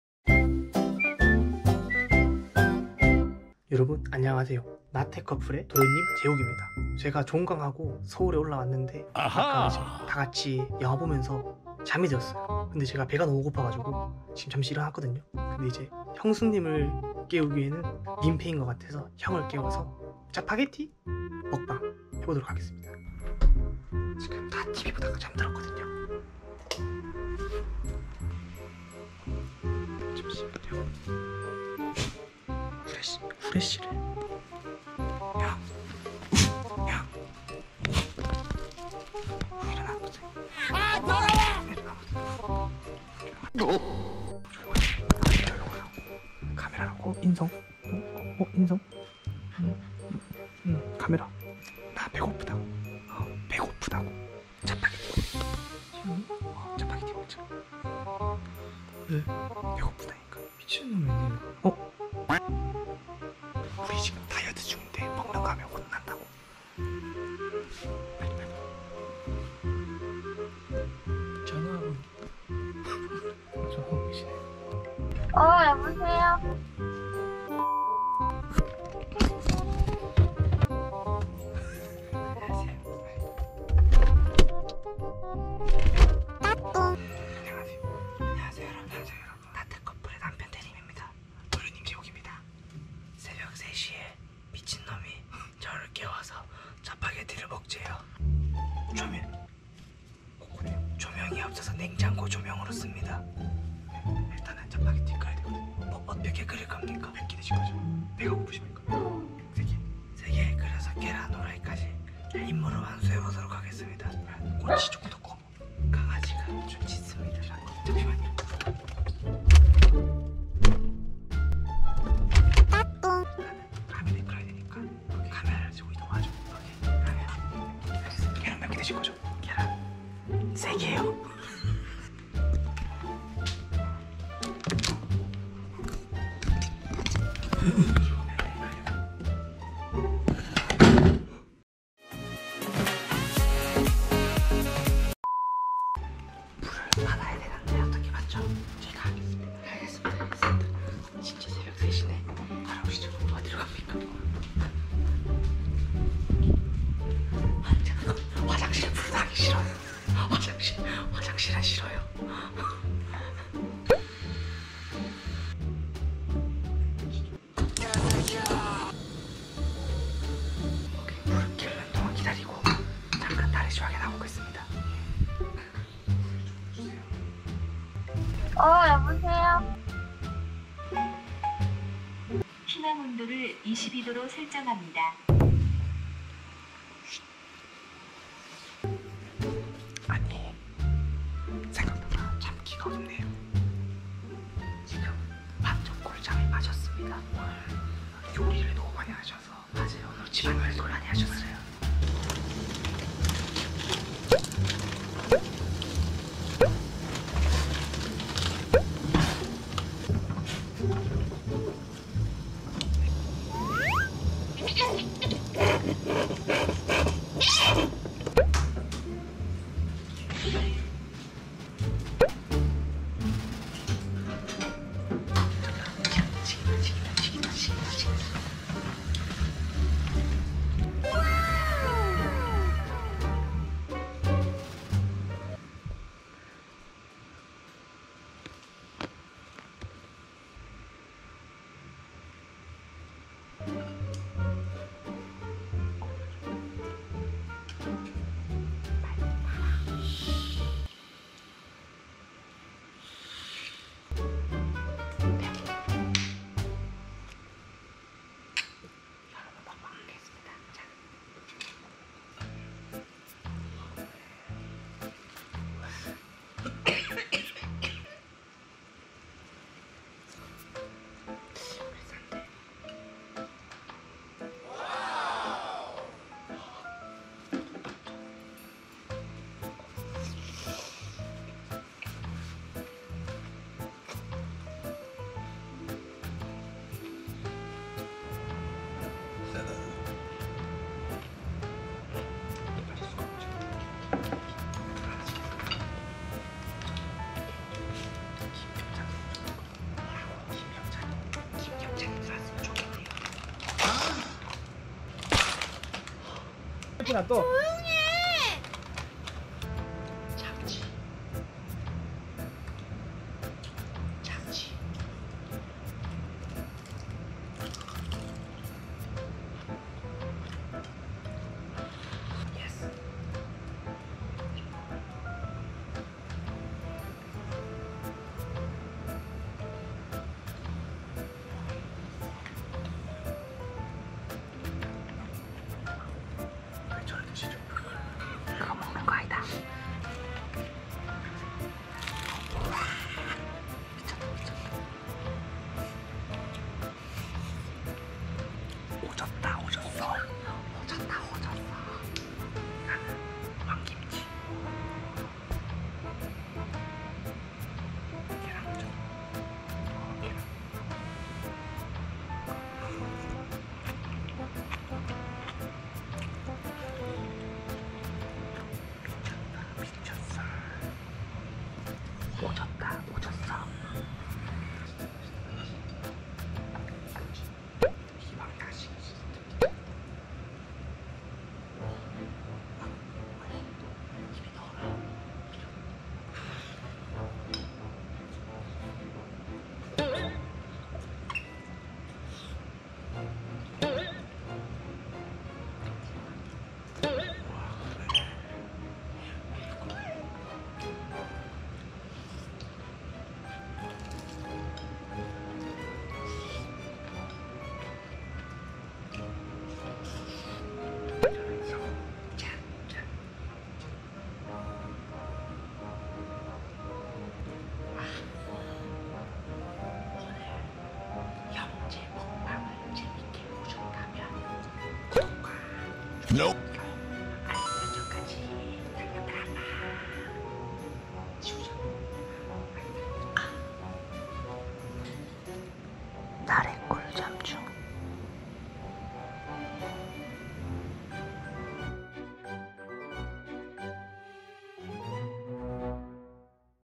SUB)다이어트 중인 형수님 잘 때 짜파게티 먹방👀 도련님 왔어요~~👶🏻
🛑나래&태림이와 함께하는 오픈 채팅🛑
     채널에 가입하여 혜택을 누려보세요.
 Instagram 인스타그램
나태커플  nt.couple
나래  nr_282
태림  t920210